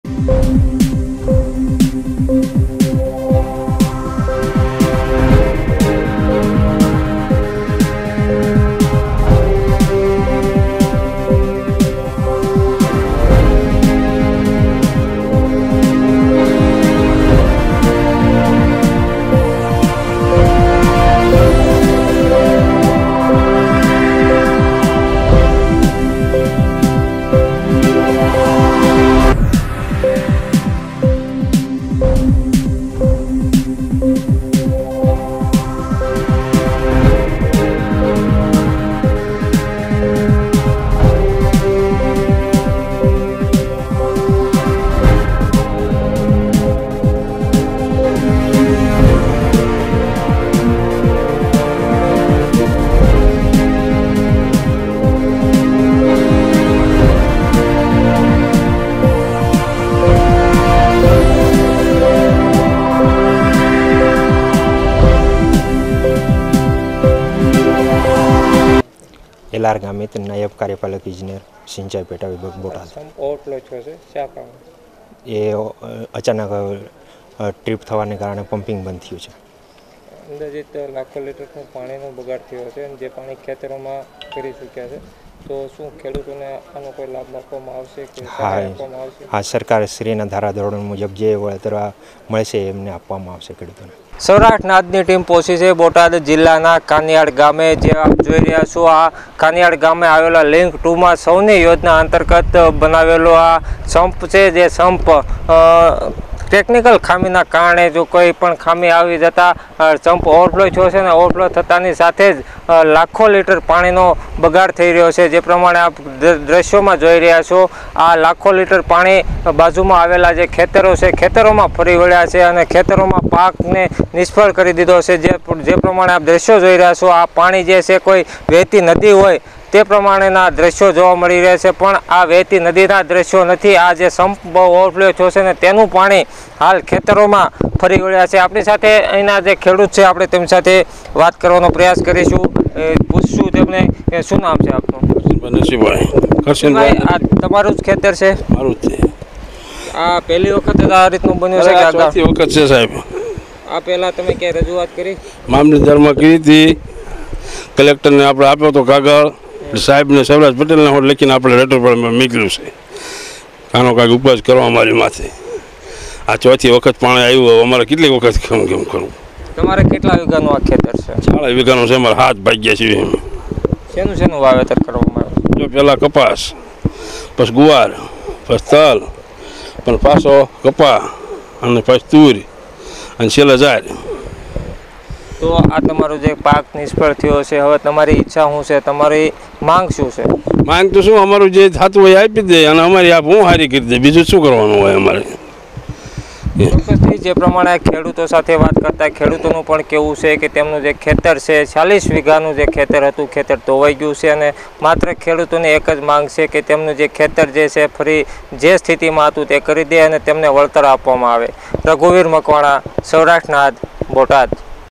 કાનીયાડ ગામે સૌનીયોજનાનો સંપ ઓવરફલો થતા લાખો લીટર પાણીનો વ્યય
સૌરાષ્ટ્રનાદ બોટાદ